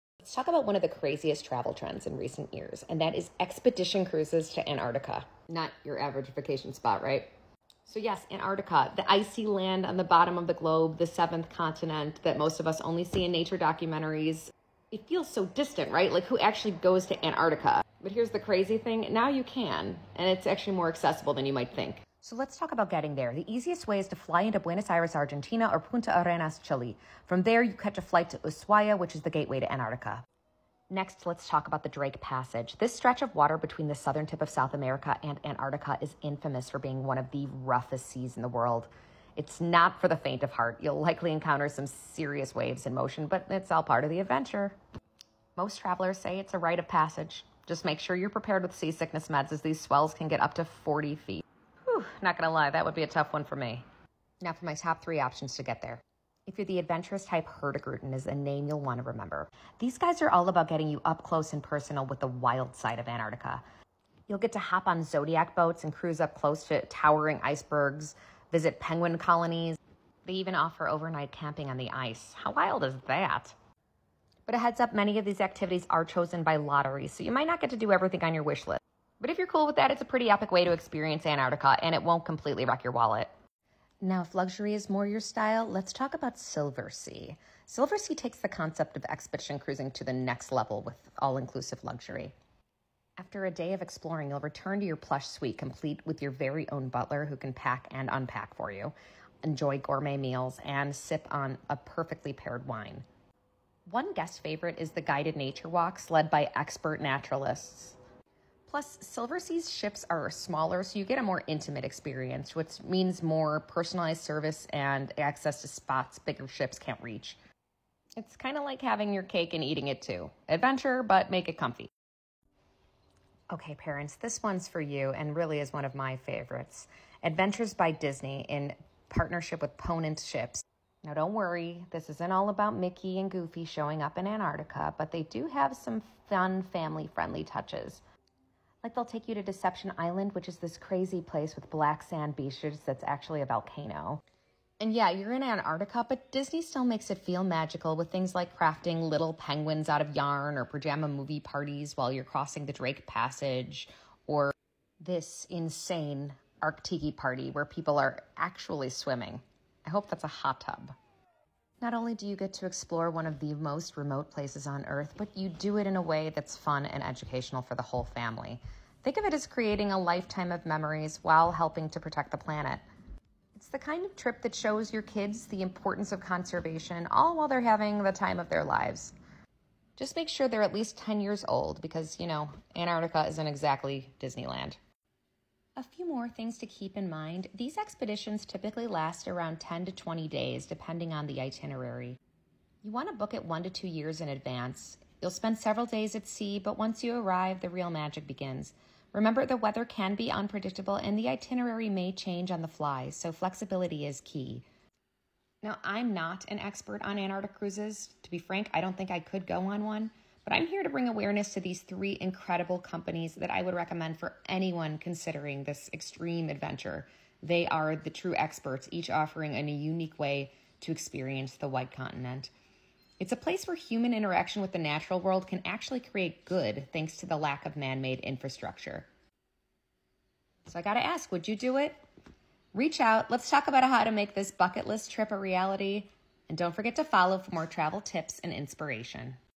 Marisa’s Antarctica Insights with - Evyssa Vacations
Marisa from our team put together an informative video on Antarctica, the world’s most isolated continent that’s more accessible than ever! Learn about the last great wilderness and the expedition cruises that take you there. Drop a comment if exploring Antarctica is on your bucket list!

@HURTIGRUTEN @Adventures by Disney @Silversea Cruises @PONANT